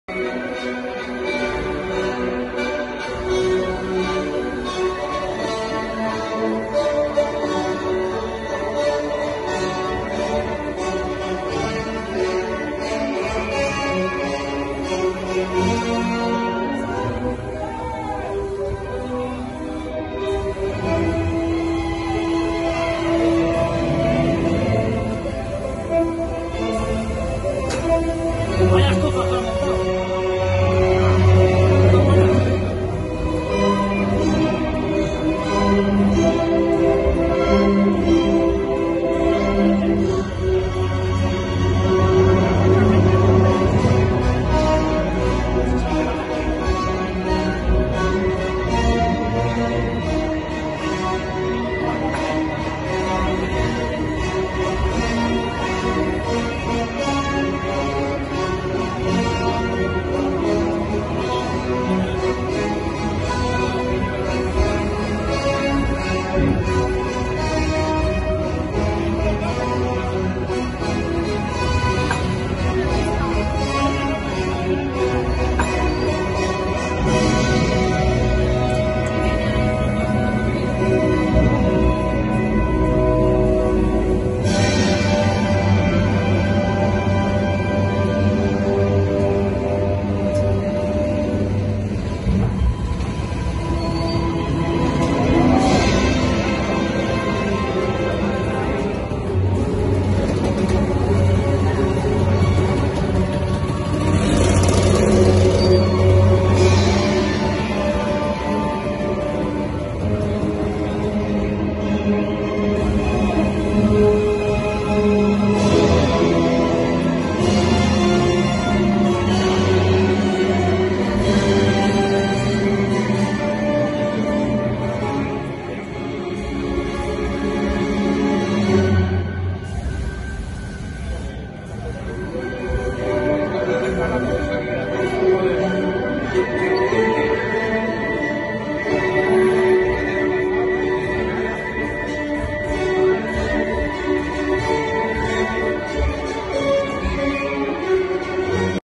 La Leyenda Continúa en Turégano 2020
Inauguración del Año Nuevo Motero en Turégano el 10 de enero de 2020